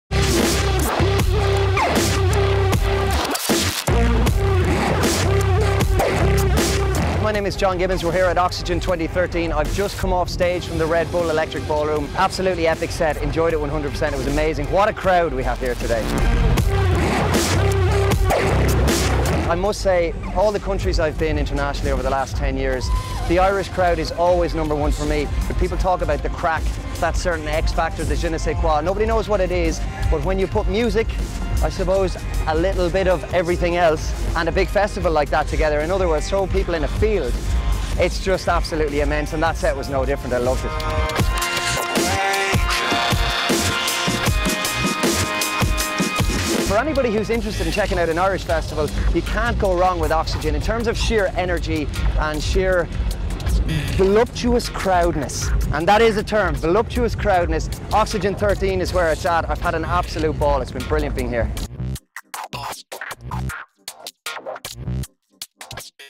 John Gibbons - Red Bull Electric Ballroom at Oxegen 2013 Ireland
John Gibbons plays to a packed house at the Red Bull Electric Ballroom. This show at Oxegen 2013 kicks off the Power tour.